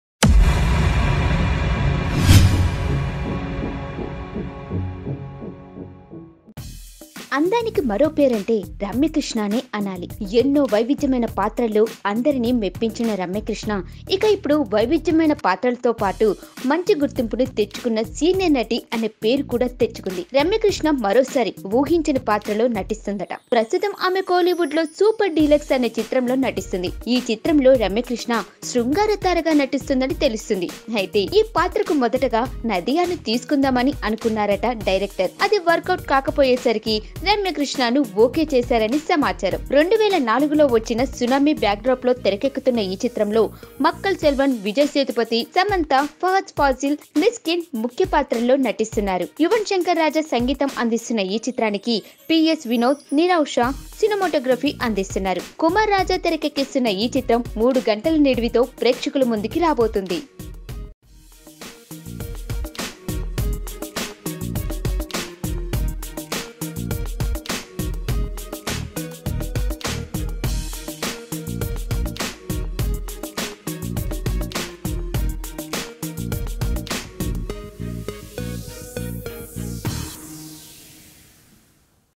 Ramya Krishnan Plays UnExpected Role in VijaySethupathi's SuperDeluxe Movie | Latest Cinema News
Watch : Ramya Krishnan Plays UnExpected Role in VijaySethupathi's SuperDeluxe Movie